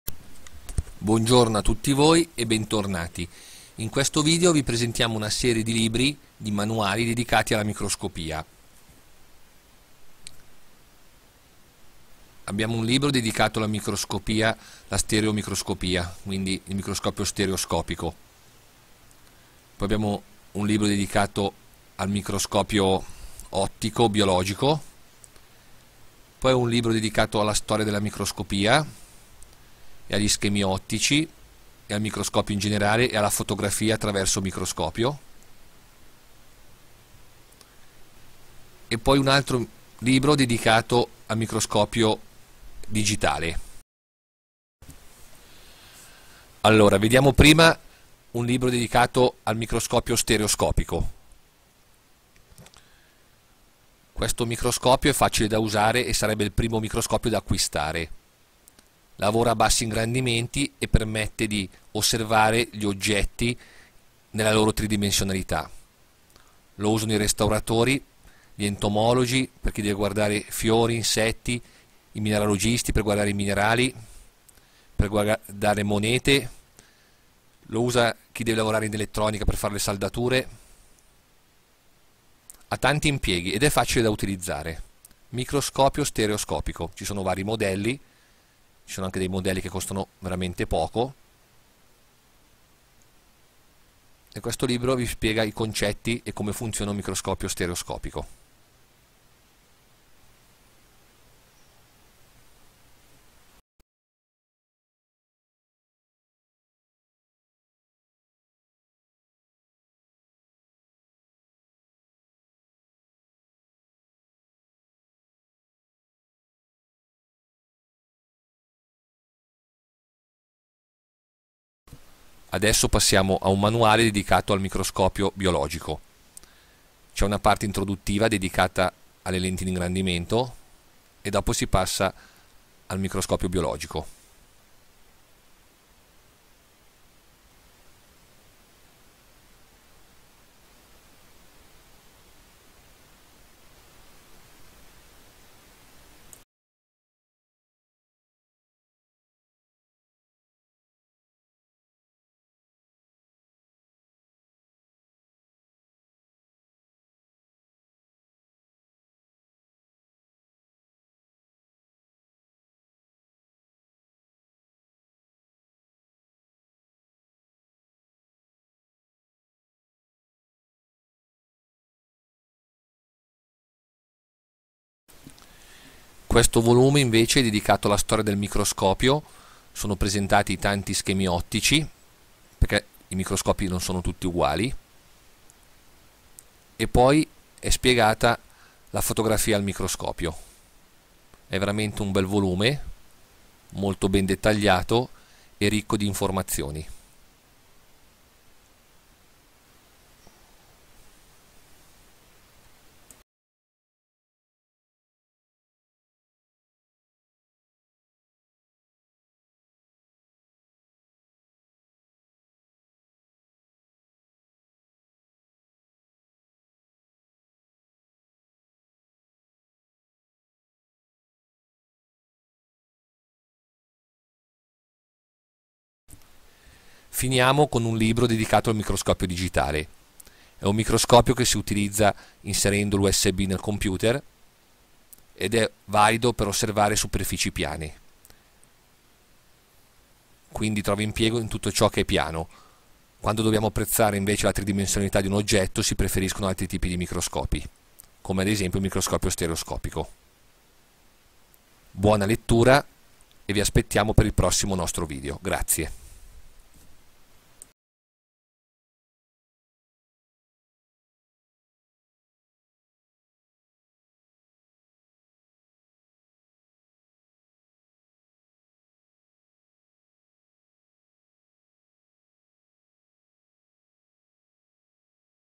Microscopia base. Utili libri dedicati al microscopio e alla microscopia. ottica pratica e utile
Alcuni libri dedicati al microscopio ed alla microscopia. Microscopio stereoscopico, microscopio biologico, microscopio digitale. La fotografia e il microscopio.
Impariamo ad usare bene questo strumento scientifico
Buona visione e buona lettura.